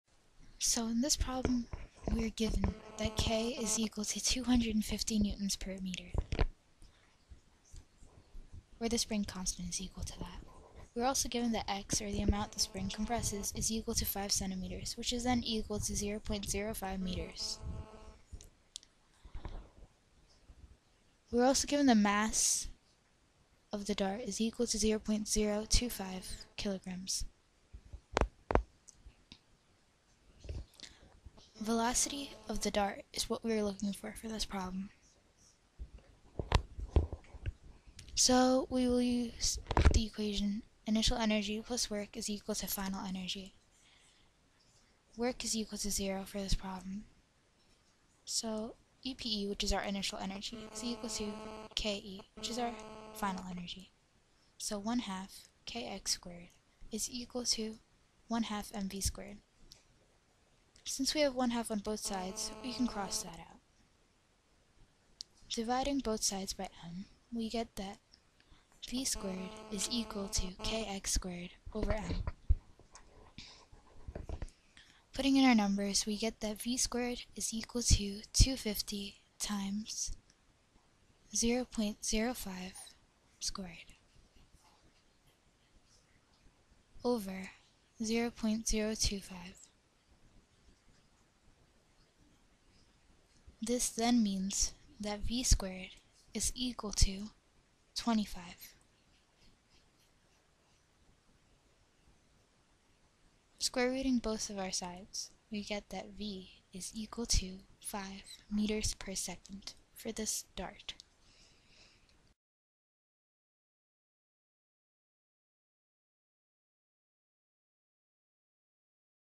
Work and Energy Presentation Slide 42
A spring gun with a spring constant of 250 N/m is compressed 5 cm. How fast will a 0.025 kg dart go when it leaves the gun?